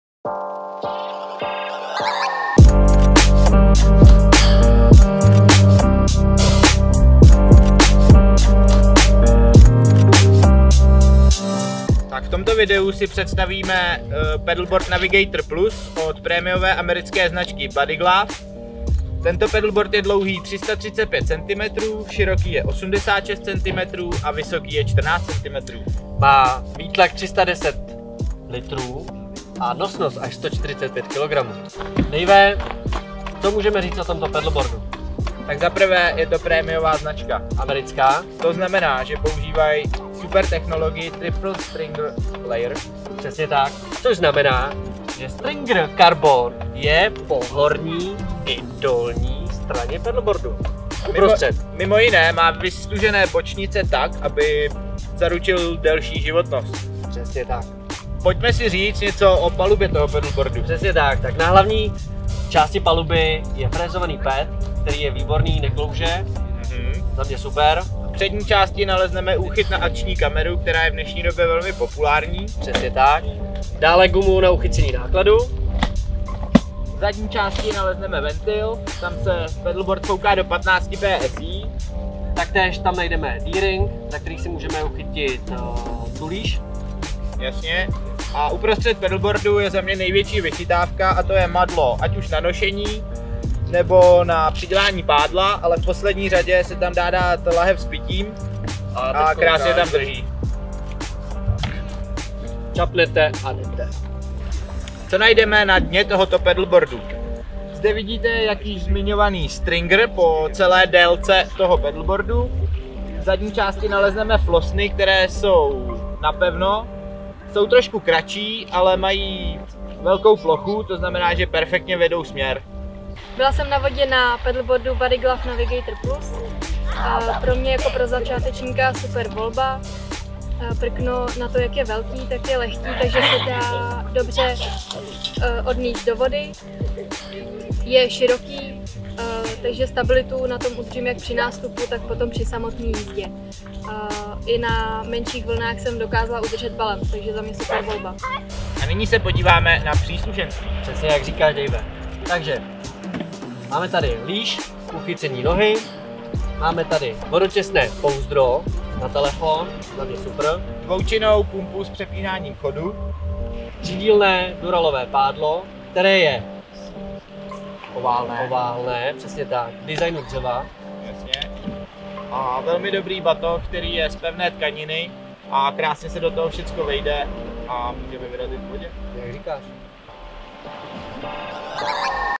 Paddleboard Body Glove Navigator 11-34
Allround paddleboard prémiové americké značky Body Glove Navigator +. Super tuhá konstrukce díky výztuhám z carbonu. Zajímavé doplňky jako jsou multifunkční madlo nebo závit na akční kameru.

Výtlak 310 litrů z něj dělá ideální rodinný paddleboard - rodič+dítě nebo pro jezdce 90 kg+.